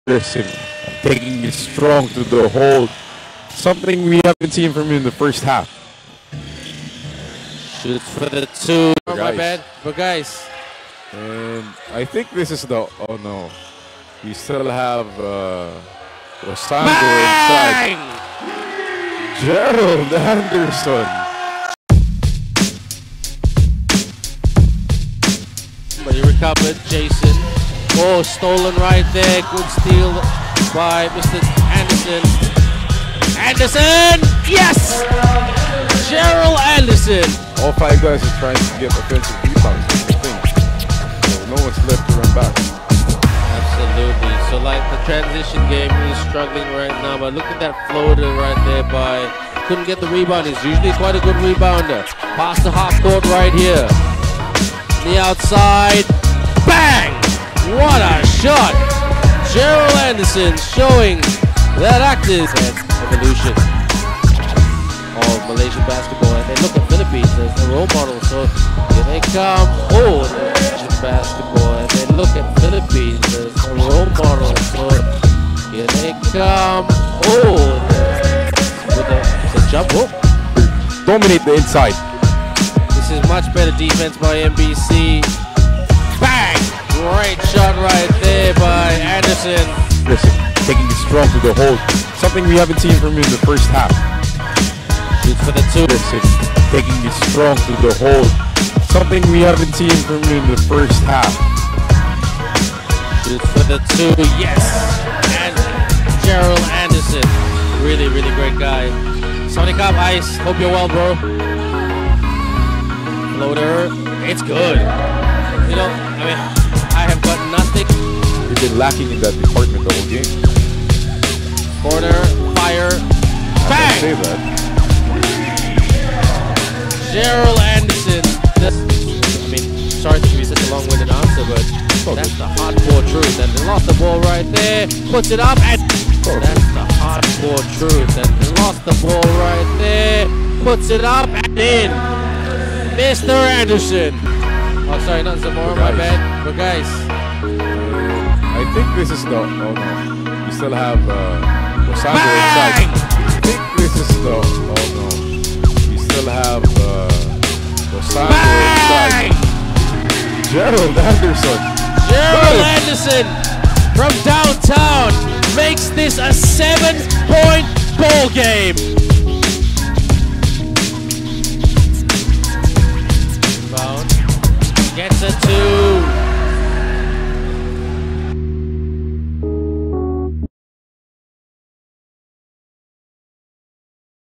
GERALD ANDERSON NAGPASIKAT SA MALAYSIA! 24 POINTS!
Gerard Anderson AsiaBasket Debut
24 pts 3 rebs 3 sts 5 stls.